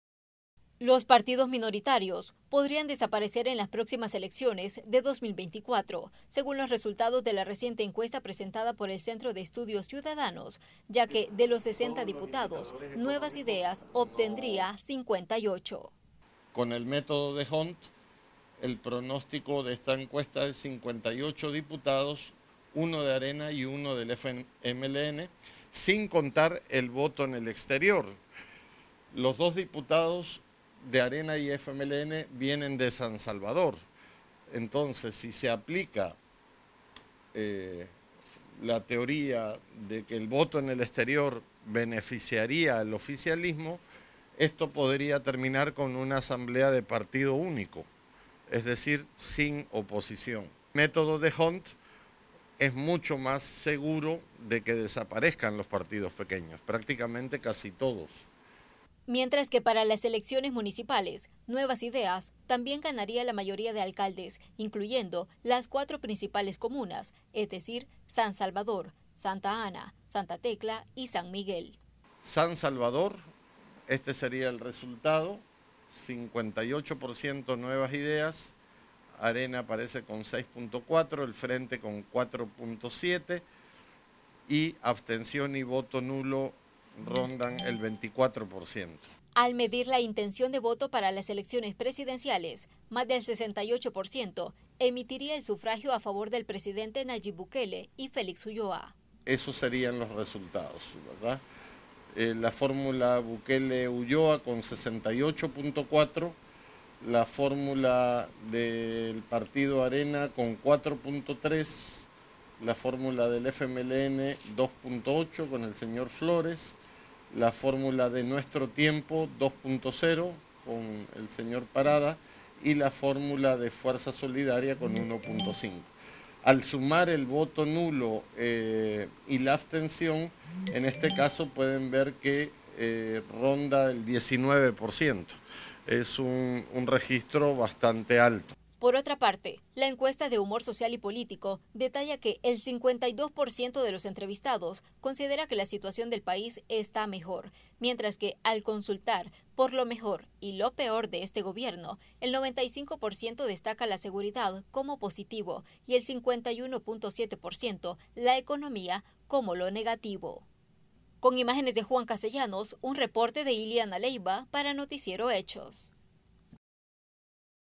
El Centro de Estudios Ciudadanos de la UFG presentó los resultados de su última encuesta
El Centro de Estudios Ciudadanos presento los resultados de su última encuesta de opinión donde destaca la intención de voto para los próximos comicios.